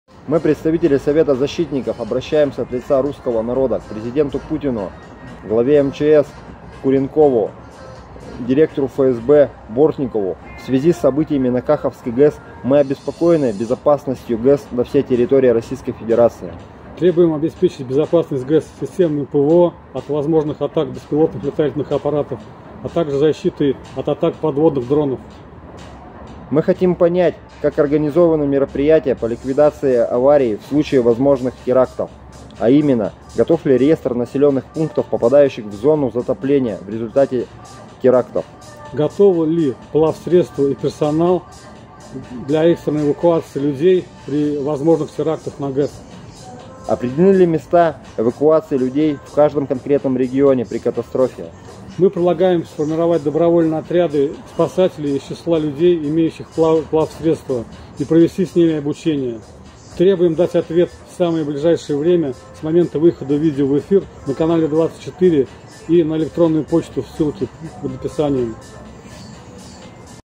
Обеспечьте безопасность Каховской ГЭС!
☑ Требование Совета Защитников к президенту РФ Путину, главе МЧС Куренкову, директору ФСБ Бортникову об обеспечении безопасности ГЭС на территории РФ.